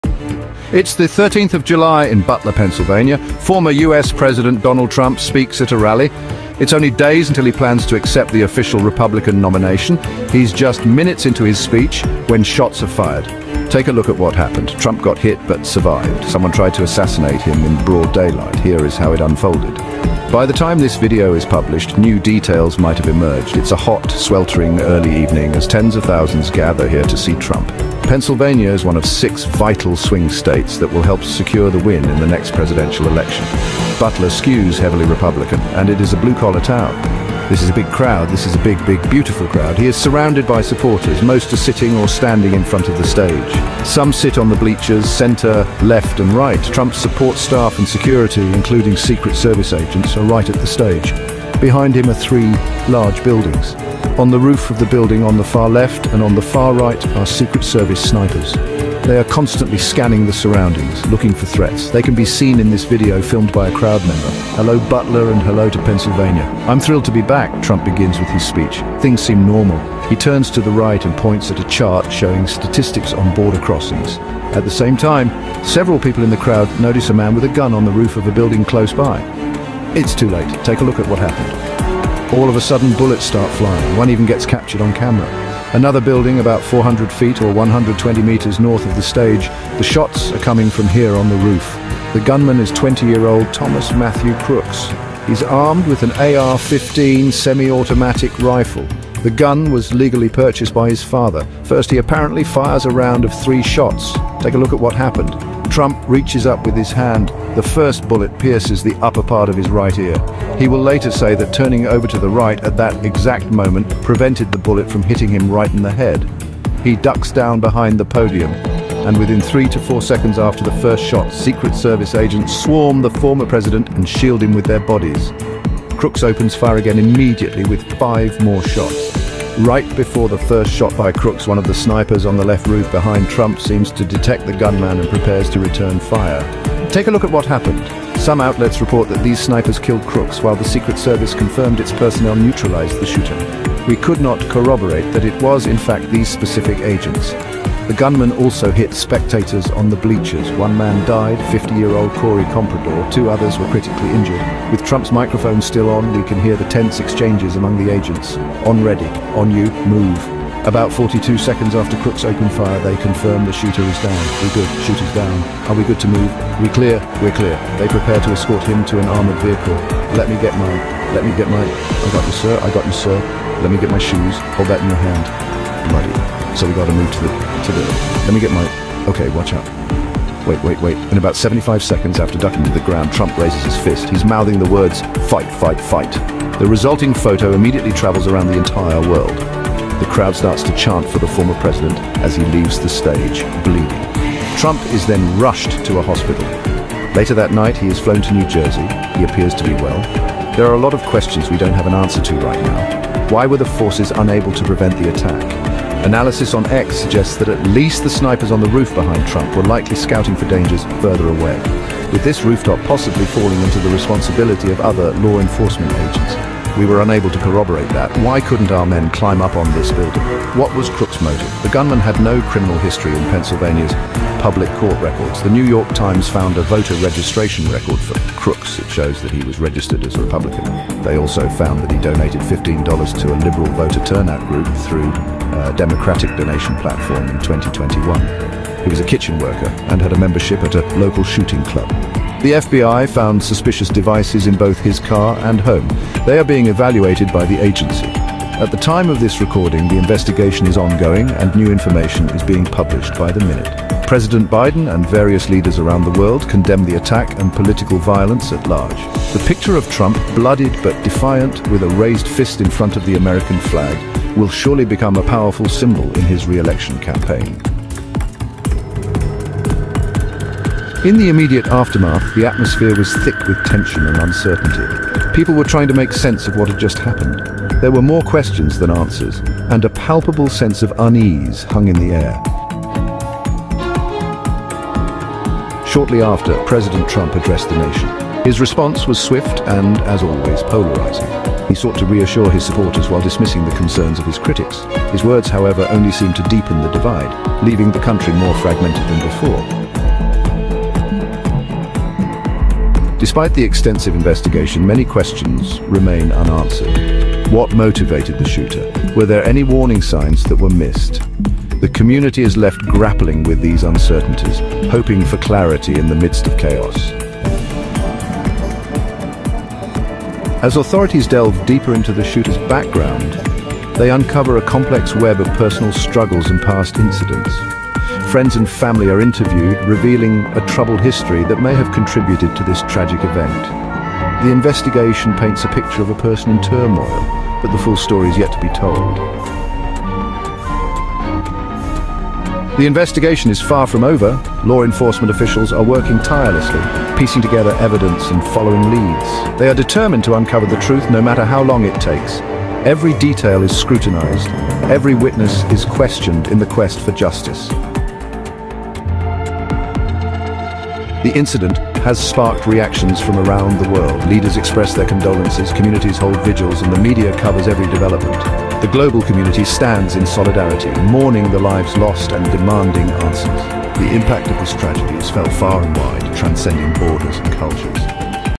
Assassination Attempt on Donald Trump / Tense Moments at the Rally
"In this video, we delve into the assassination attempt on former US President Donald Trump during a rally. We share footage from the event, the tense moments that unfolded, and how Trump survived the attempt. Watch our video to explore the turbulent political landscape and the background of this assassination attempt in detail.

00:00:00 Introduction

00:04:30 The Rally Begins

00:04:30 The Assassination Attempt

00:04:30 The Shooter

00:04:30 Immediate Aftermath

00:04:45 Trump's Response

00:05:02 Unanswered Questions

00:05:19 Shooter's Background

00:05:42 Ongoing Investigation

00:06:00 Global Reactions